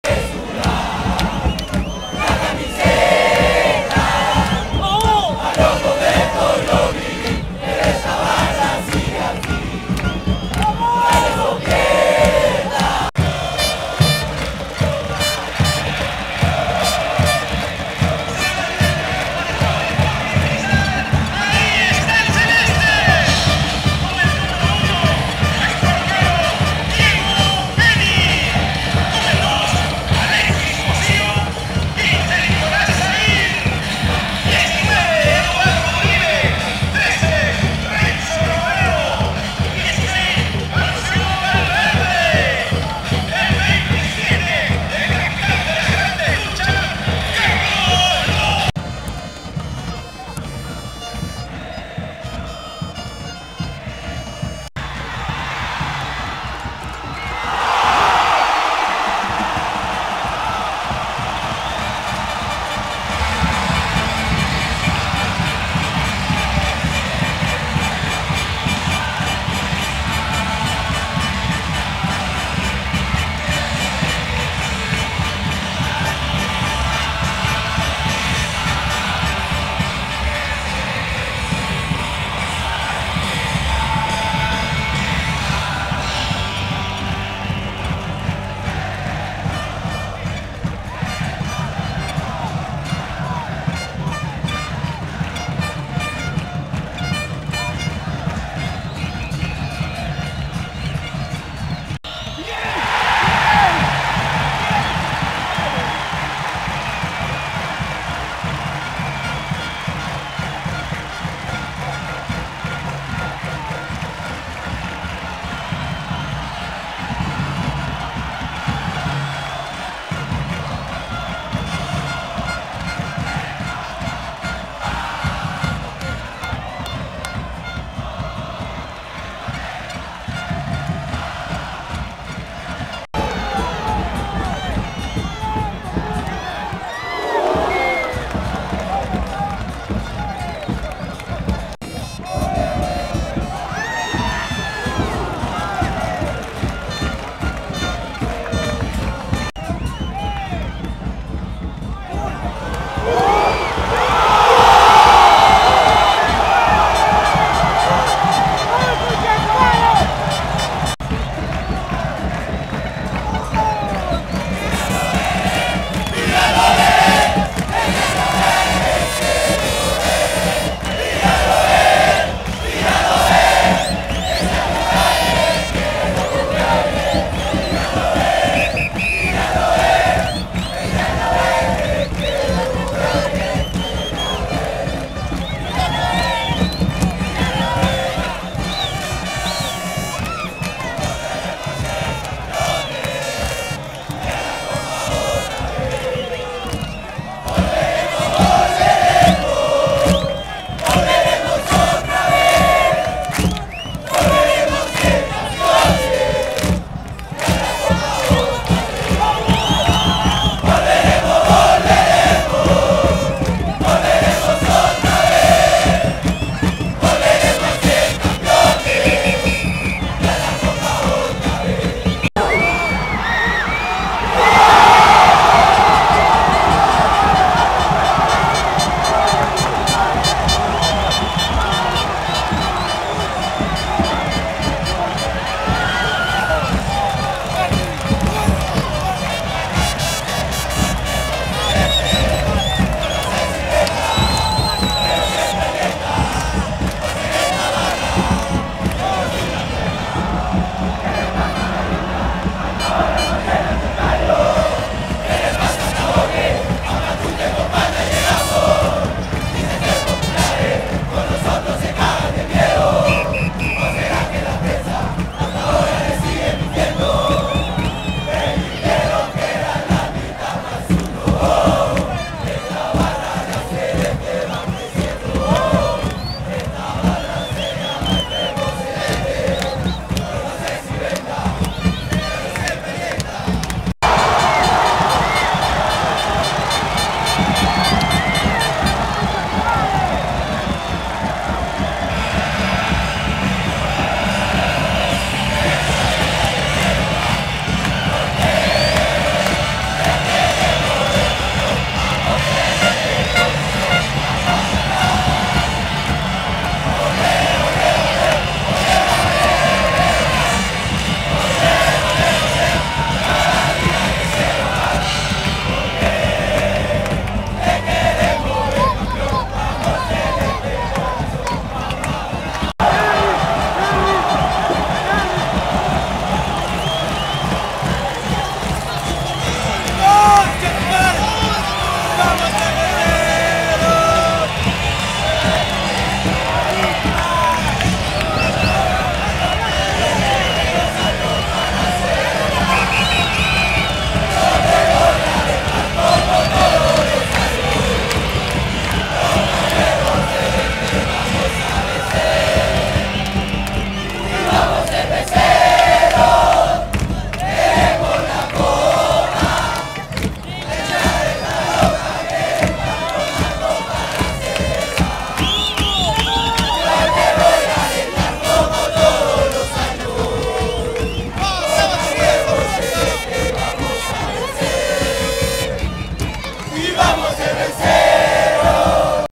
Sporting Cristal 5 vs. Pacifico 0 (27-10-2013). Fecha No. 39 del Torneo Peruano.
Sporting Cristal 5 vs. Pacifico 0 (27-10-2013).
El Portal Celeste desde cancha del Estadio Alberto Gallardo.
Fecha No. 39 del Torneo Peruano.